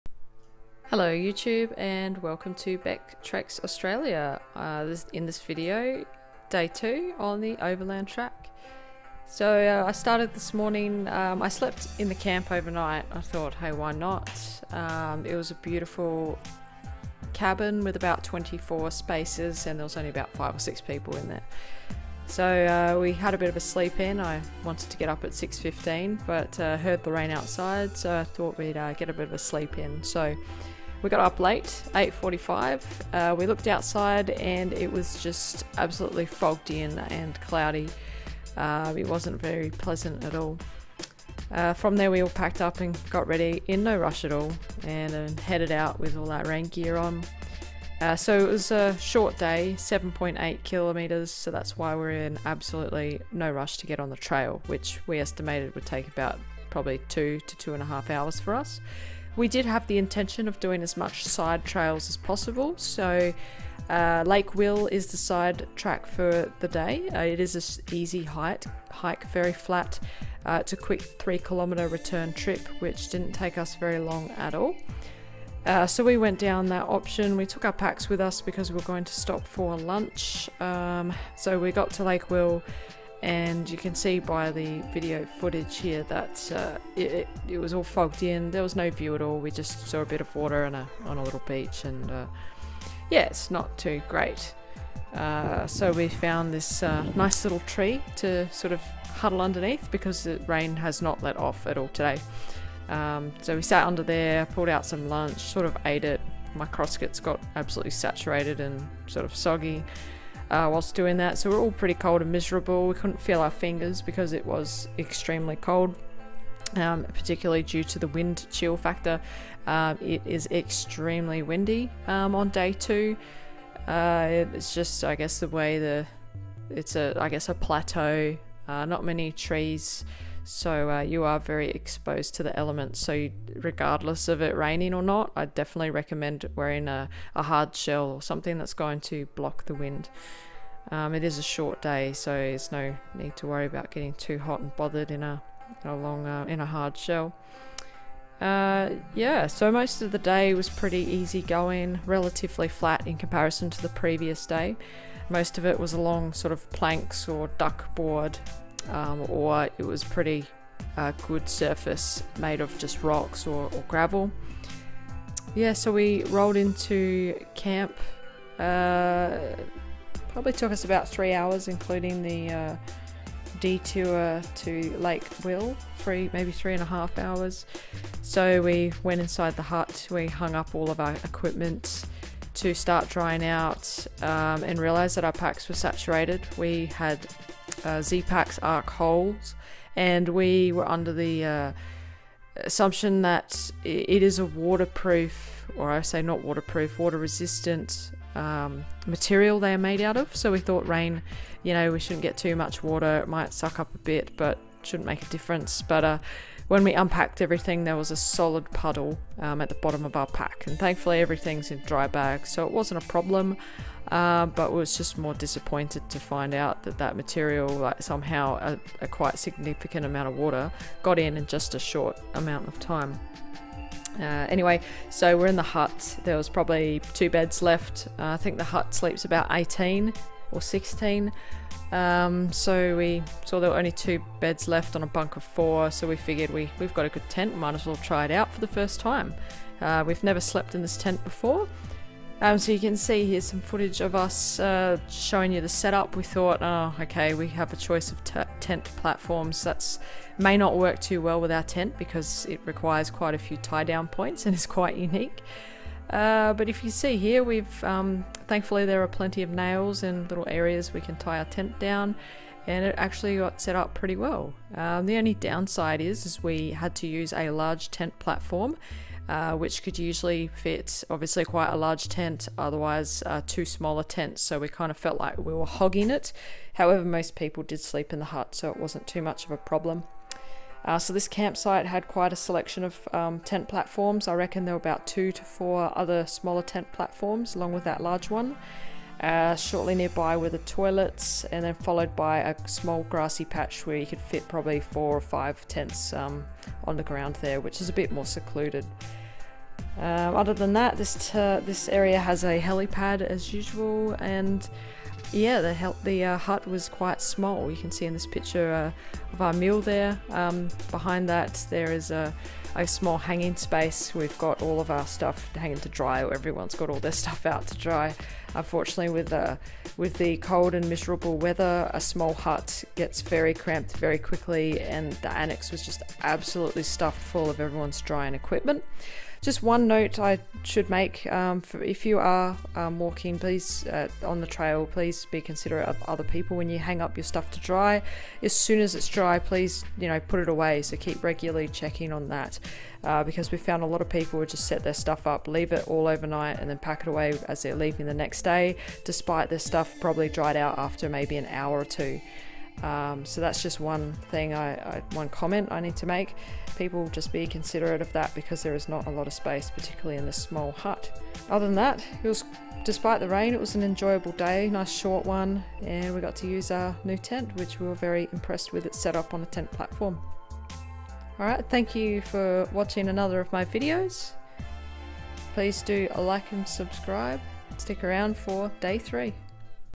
Overland Track - Cradle Mountain to Lake St. Clair - Day 2
Waterfall Valley Hut to Windermere Hut, including a quick look at how the Zpacks Triplex tent went setup on a tent platform!

An overview of how the 2nd day on trail went. Unfortunatly the weather was not the best, however we were able to make the most of it and enjoyed the scenery that was not hidden by cloud!